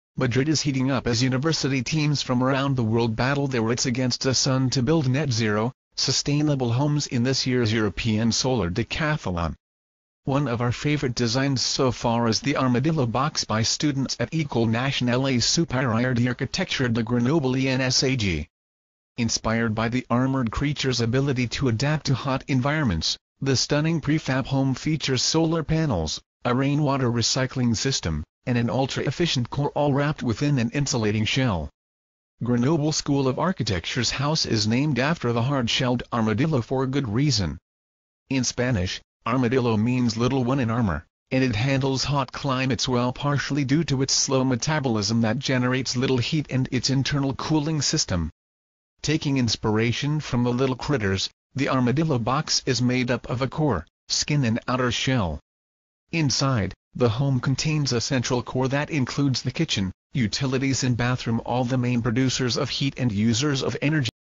Armadillo Box Solar-Powered Prefab Gears up For Solar Decathlon
Best Sellers in Tools & Home Improvement

Best Sellers in Amazon Devices & Accessories

Armadillo Box Solar-Powered Prefab Gears up For Solar Decathlon "Madrid is heating up as university teams from around the world battle their wits against the sun to build net-zero, sustainable homes in this year’s European Solar Decathlon. One of our favorite designs so far is the Armadillo Box by students at École Nationale Supérieure d’Architecture de Grenoble (ENSAG). Inspired by the armored creature’s ability to adapt to hot environments, this stunning prefab home features solar panels, a rainwater recycling system, and an ultra-efficient core all wrapped within an insulating shell.

Grenoble School of Architecture’s house is named after the hard-shelled armadillo for good reason. In Spanish, armadillo means “little one in armour”, and it handles hot climates well partially due to its slow metabolism that generates little heat and its internal cooling system. Taking inspiration from the little critters, The Armadillo Box is made up of a core, skin and outer shell. Inside, the home contains a central core that includes the kitchen, utilities and bathroom — all the main producers of heat and users of energy. The skin is a low-tech, low-cost frame with a super tight insulation made from local, inexpensive materials. Finally the outer shell serves to protect the home’s interior home from the elements, offering shade, natural ventilation and renewable energy generation via solar panels on the roof.